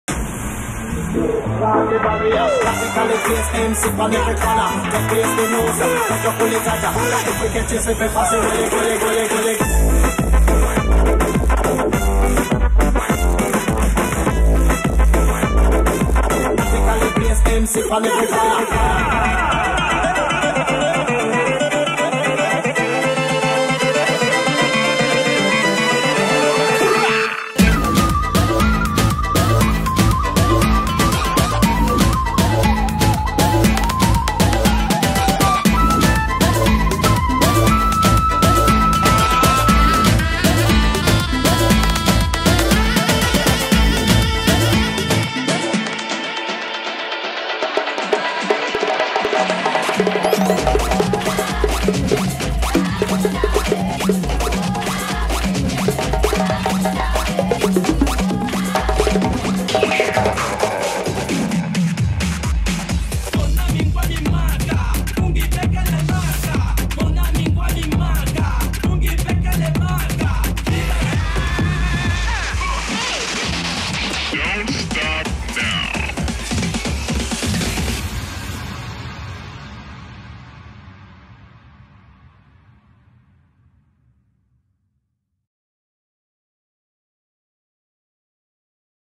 Tactical Groove Orbit 2015
Un vent digital nouveau souffle sur les musiques du monde et disperse les codes de l’électro-world.
Dans le labo de Tactical Groove Orbit palpitent les sonorités des nations, réchauffées par le feu de la "global bass".
Et c'est un voyage vibrant qui commence, des confins des territoires balkaniques aux plages brésiliennes, des pubs irlandais aux music-clubs de la Nouvelle-Orléans : électro-ska, salsa, new cumbia, bollywood grooves, digital blue-grass, balkan beat, électro surf... Le son est multiple et le dosage de ses mélanges unique : thèmes traditionnels colorés, basses puissantes et maîtrisées, leads tendus, breaks taillés au sabre.
Sur les planches, le spectacle prend sa pleine lumière : Tactical redessine là encore les contours du live-act, soumet ses machines et caresse d’étranges instruments hybrides comme sa musique, mélodieux et vivants comme son univers.
Vous êtes à bord d’un train à destination unique : Partout! Bon voyage.